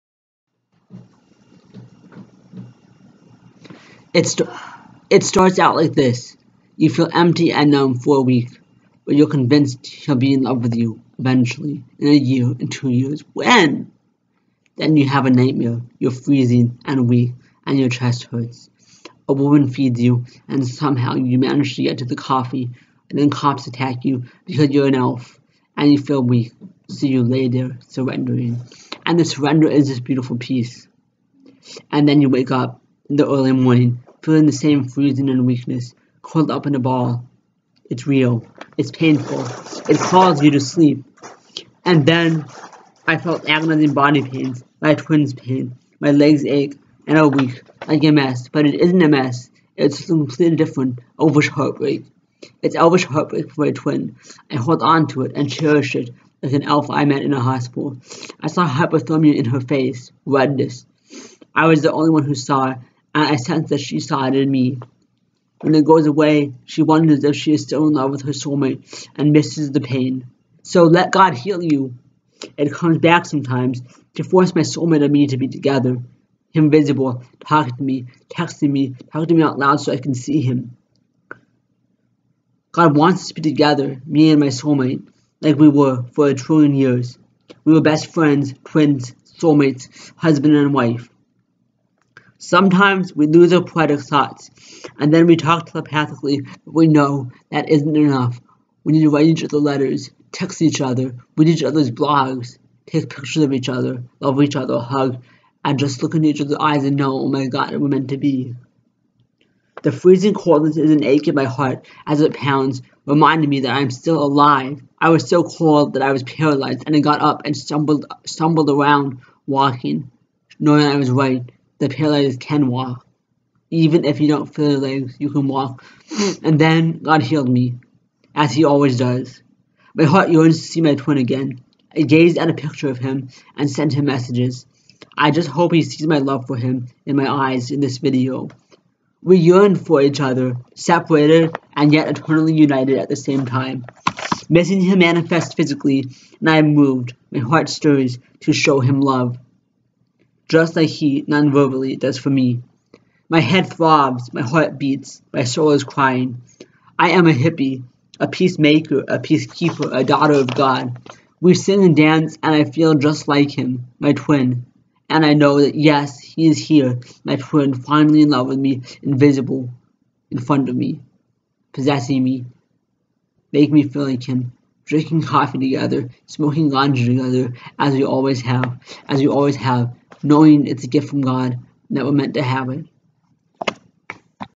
Don't lose your elfhood: slam poetry
A poem about elvish heartbreak I wrote. SO. you don't have togive up on that person you are madly in love with, as long as they are the opposite gender. DON'T GIVE UP. Pray and believe. I really think it's gonna happen. then maybe it will.